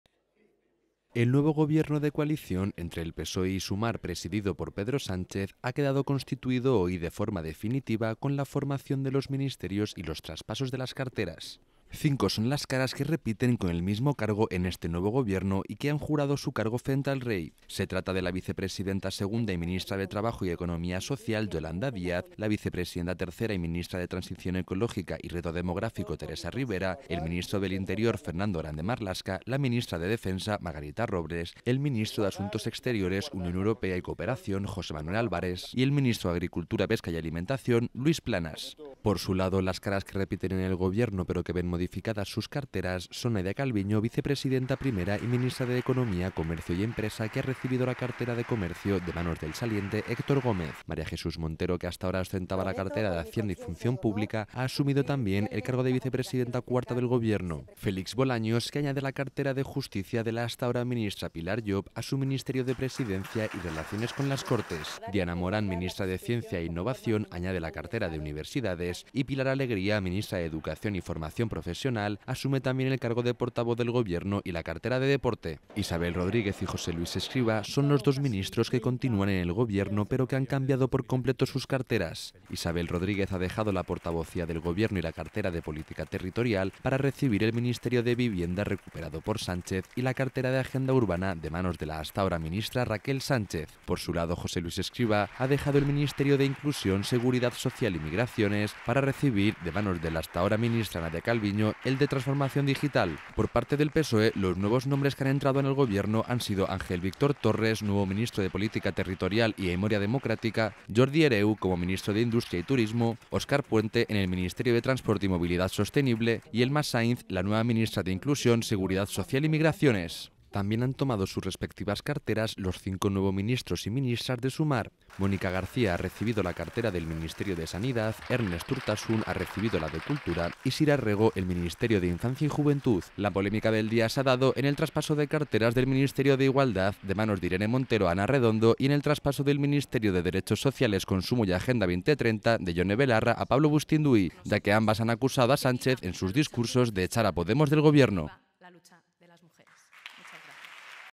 Así queda el nuevo Gobierno de Pedro Sánchez tras asumir el cargo todos los ministros y ministr
El nuevo Gobierno de coalición entre el PSOE y Sumar presidido por Pedro Sánchez ha quedado constituido hoy de forma definitiva con la formación de los ministerios y los traspasos de las carteras.
Cinco son las caras que repiten con el mismo cargo en este nuevo Gobierno y que han jurado el cargo frente al Rey, se trata de la vicepresidenta segunda y ministra de Trabajo y Economía Social, Yolanda Díaz; la vicepresidenta tercera y ministra de Transición Ecológica y Reto Demográfico, Teresa Ribera; el ministerio del Interior, Fernando Grande-Marlaska; la ministra de Defensa: Margarita Robles; el ministro de Asuntos Exteriores, Unión Europea y Cooperación, José Manuel Albares; y el ministro de Agricultura, Pesca y Alimentación, Luis Planas.
Por su lado las caras que repiten en el Gobierno pero que ven modificadas sus carteras son Nadia Calviño, Vicepresidenta primera y ministra de Economía, Comercio y Empresa que ha recibido la cartera de Comercio de manos del saliente Héctor Gómez. María Jesús Montero, que hasta ahora ostentaba la cartera Hacienda y Función Pública, ha asumido también el cargo de vicepresidenta cuarta del Gobierno. Félix Bolaños que añade la cartera de Justicia de la hasta ahora ministra Pilar Llop a su Ministerio de Presidencia y Relaciones con las Cortes. Diana Morant, ministra de Ciencia e Innovación añade la cartera de Universidades. y Pilar Alegría, ministra de Educación y Formación profesional que ha asumido el cargo de portavoz del Gobierno y la cartera de Deporte.
(Fuente: Europa Press / Moncloa)